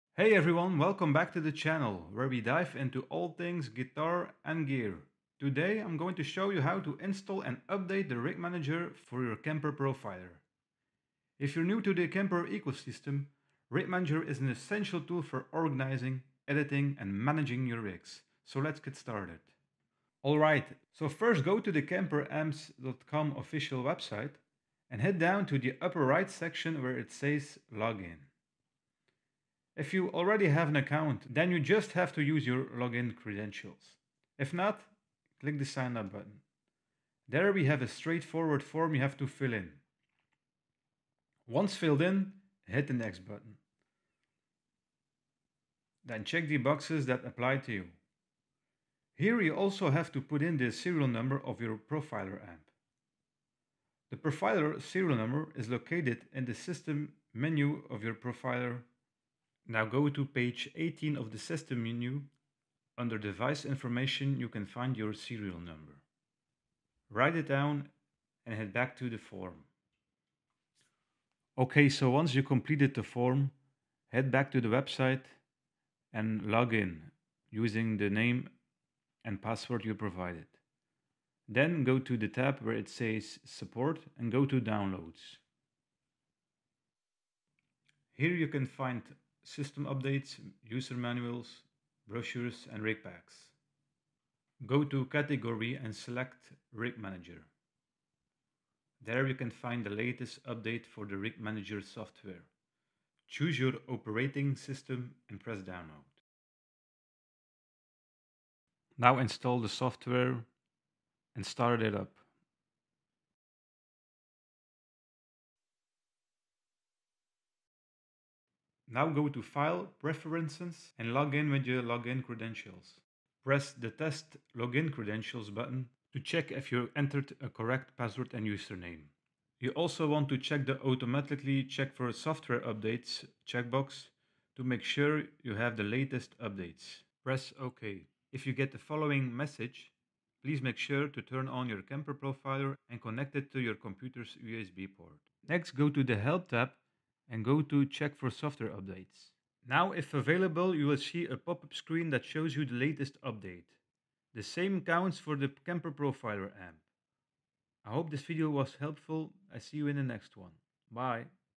Kemper Profiler Rig Manager: Install & Update Guide
Learn how to install Rig Manager & Update your kemper profiler amp and unlock its full potential! In this tutorial, we'll guide you through the step-by-step process of installing Rig Manager on your pc, allowing you to access a vast library of rigs, edit and customize your tones, and take your guitar playing to the next level. Whether you're a beginner or an experienced musician, this video is perfect for anyone looking to get the most out of their Kemper amp. So, what are you waiting for? Install Rig Manager now and start exploring the endless possibilities of your Kemper amp!
🎸 What You'll Learn:

✅ Where to download Rig Manager
✅ How to update rig manager
✅ how to update  kemper profiler
✅ Step-by-step installation instructions

Got questions or need further help? Drop them in the comments below! 🎶